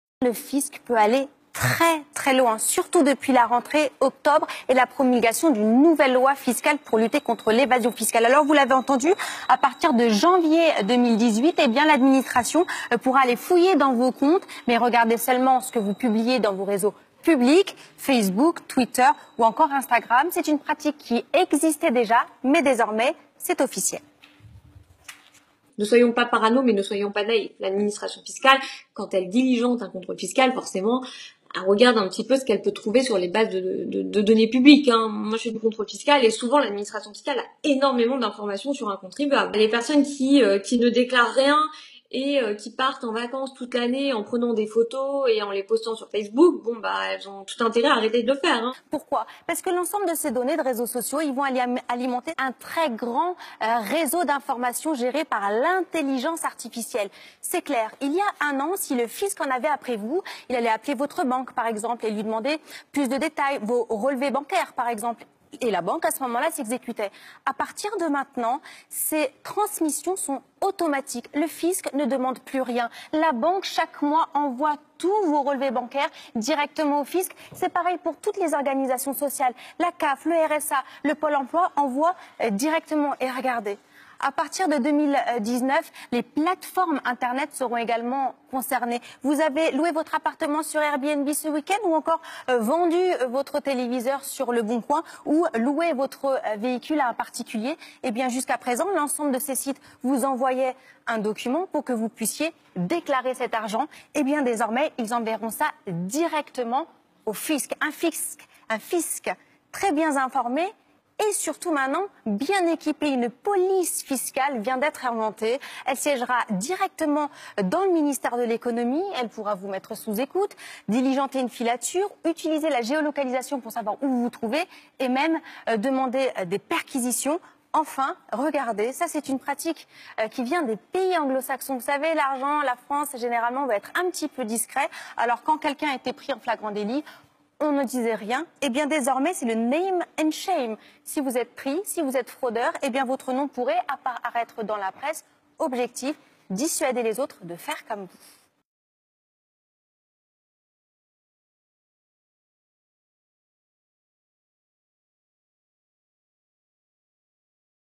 Le fisc traque les citoyens sur le net
Drôle de méthode de recouvrement du fisc en France.